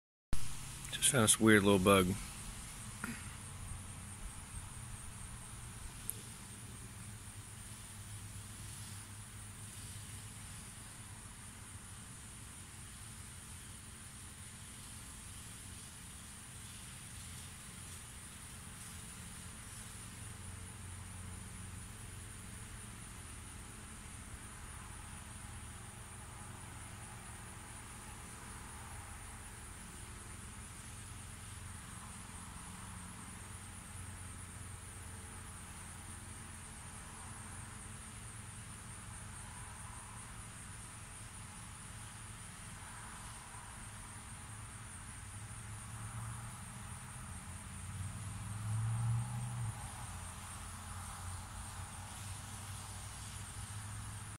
Trash Bug
Found this little dude in the backyard.

"Definition of trash bug. : an aphis lion that is the larva of a lacewing of the family Chrysopidae and that piles debris on its back."